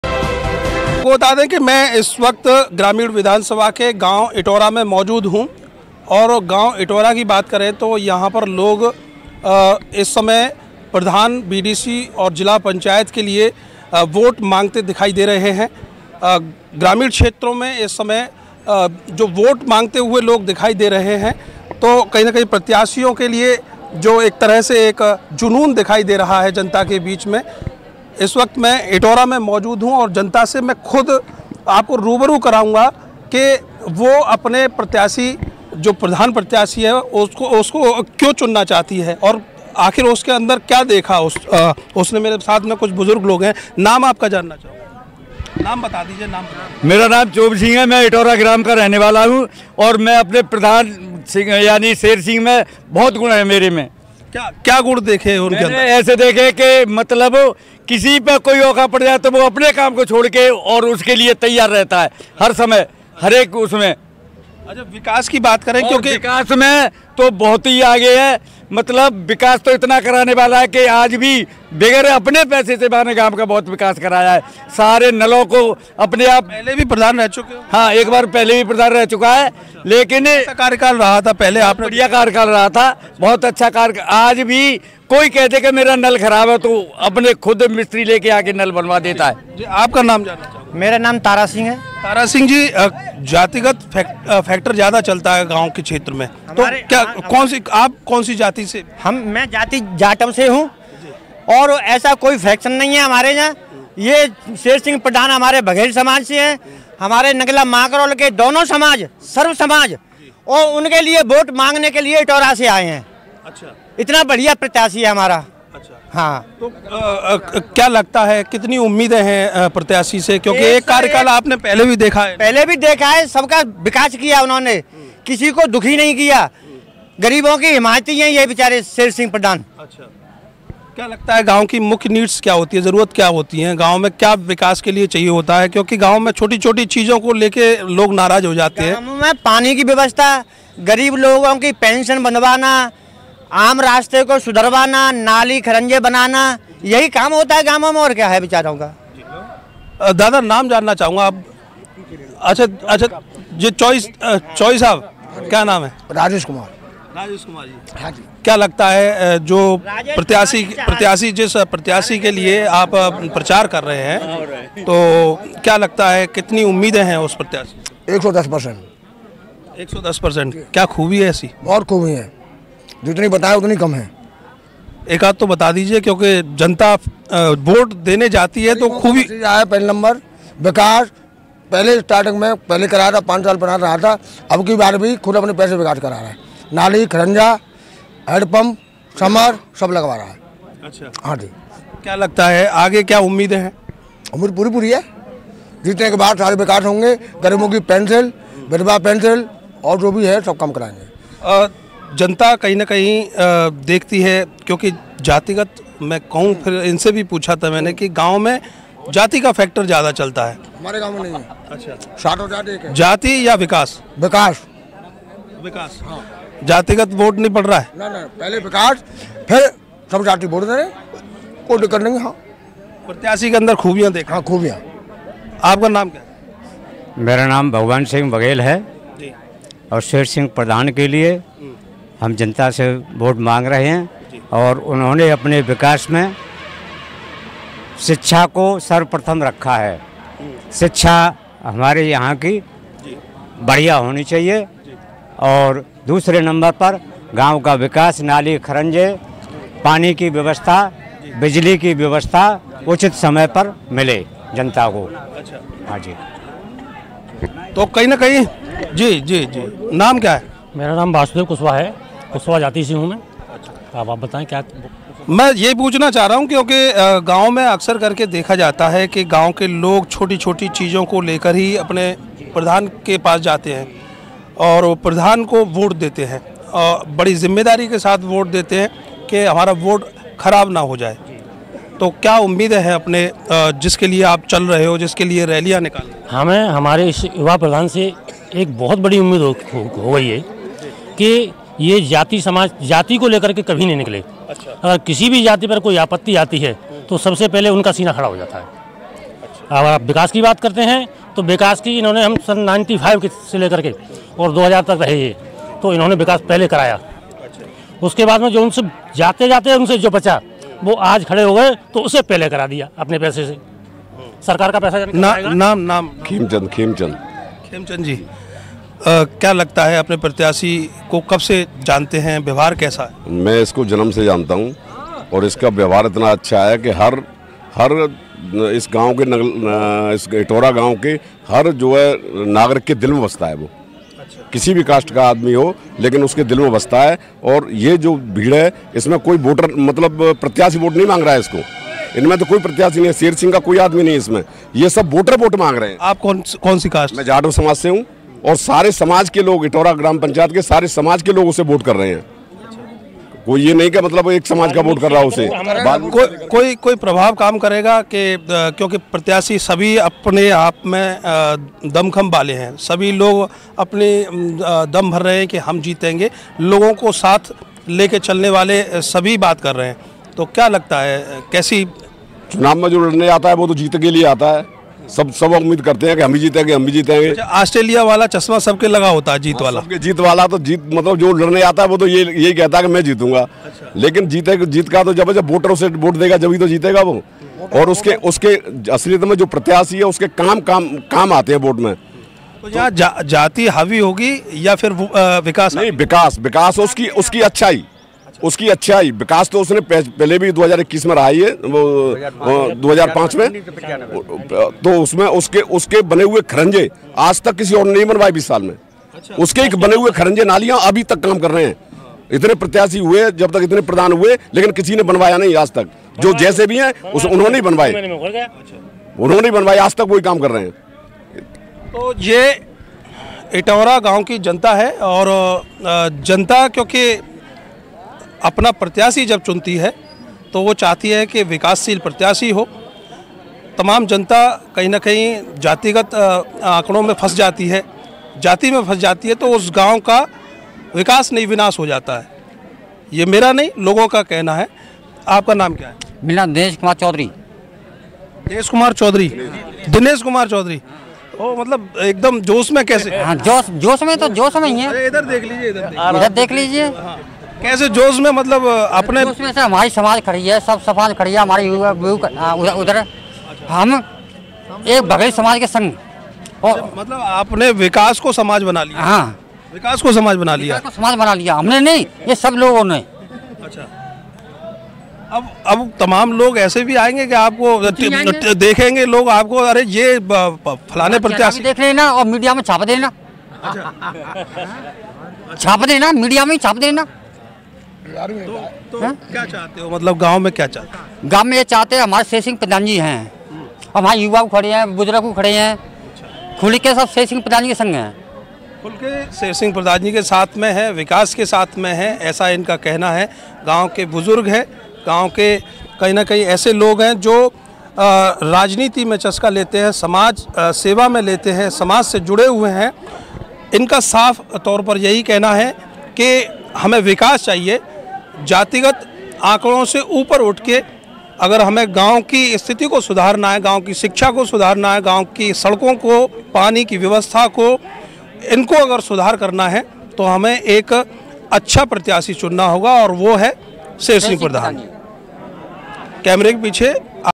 इटोरा_मैं_जनता_कर_रही_प्रधान_प्रत्याशी_शेर_सिंह_बघेल का प्रचार,जातिवाद नहीं विकासवादी_को_लगेगी_मुहर
[12:29, 4/8/2021] आगरा इटोरा गांव में प्रधान प्रत्याशी के लिए जनता ने किया जोरदार प्रचार
गांव की जनता ने मांगे प्रधान प्रत्याशी शेर सिंह के लिए वोट
कार के निशान पर वोट डालने की अपील
जातिवाद से ऊपर उठकर विकासशील के लिए लगेगी मुहर बोली जनता